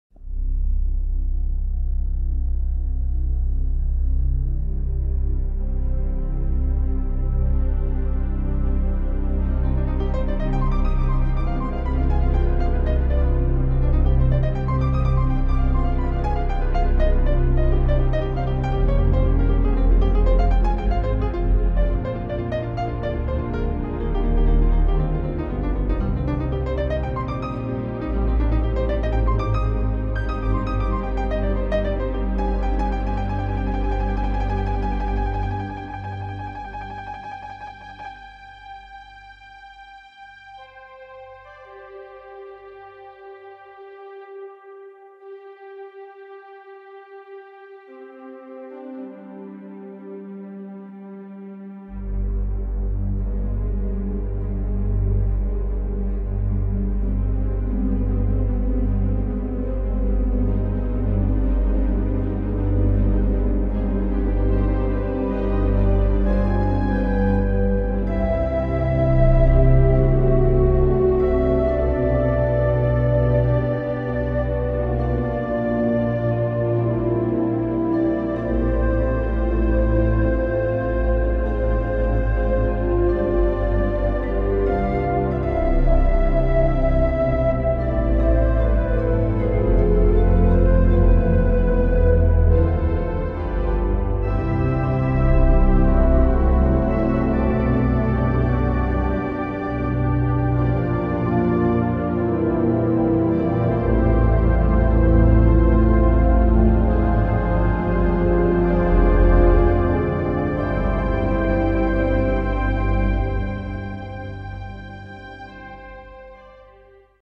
Johannus Introduction movie for the new Symphonica
Johannus introduces the new Symphonica 35: combined traditions, lifelike sounds.

Music is played live by Andre van Vliet.